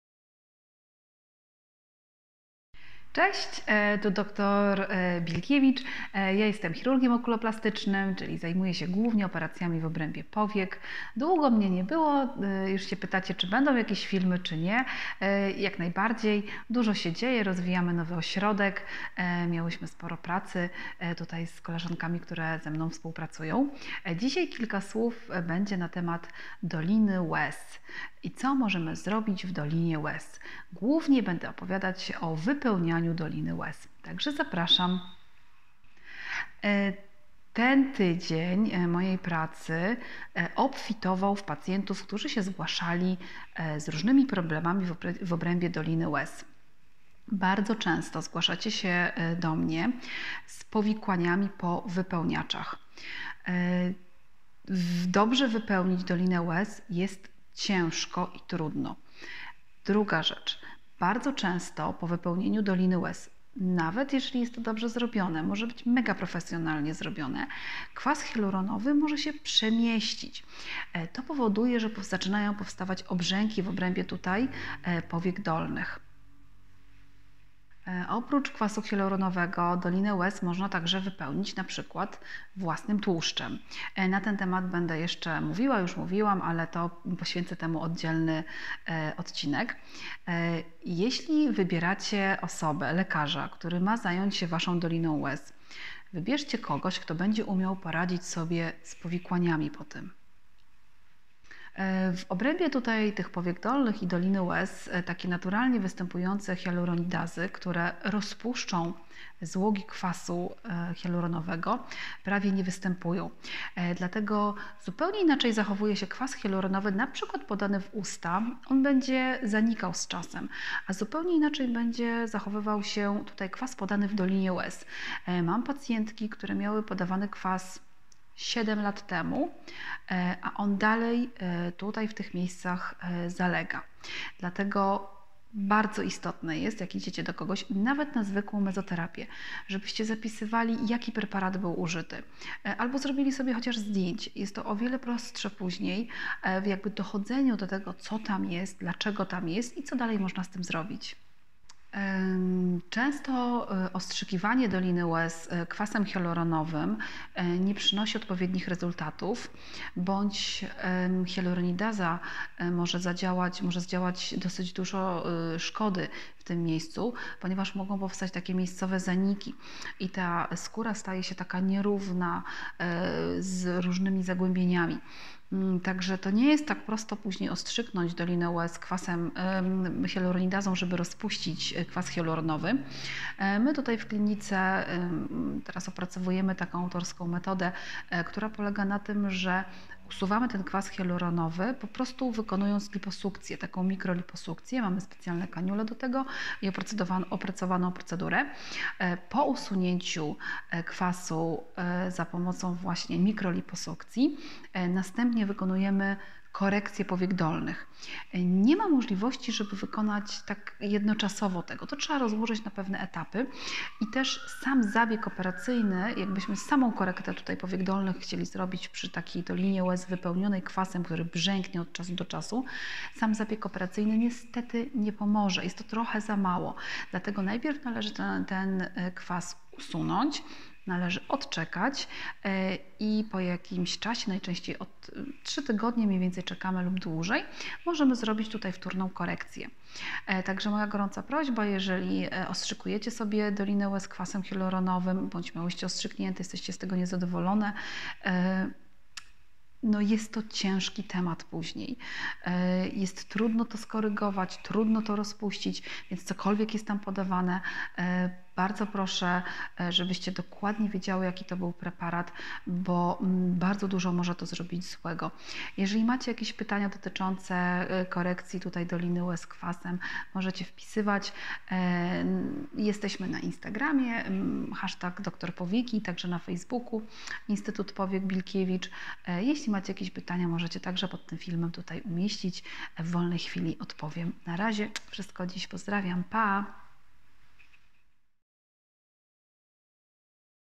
💎 WYPEŁNIANIE DOLINY ŁEZ - jak nie narobić sobie kwasu?
👁 Czy wypełnianie doliny łez kwasem hialuronowym jest bezpieczne?
👁 Do jakich powikłań może prowadzić wypełnianie doliny łez?
👁 Czy można wypełnić dolinę łez przed zabiegiem plastyki powiek dolnych?

O tym opowiem w powyższym filmie, który powstał dzięki Waszym pytaniom! 😊

Jak mnie znaleźć?

PS. Jeżeli chcielibyście, abym nagrała film na konkretny temat, to wpisujcie je w komentarzach na dole.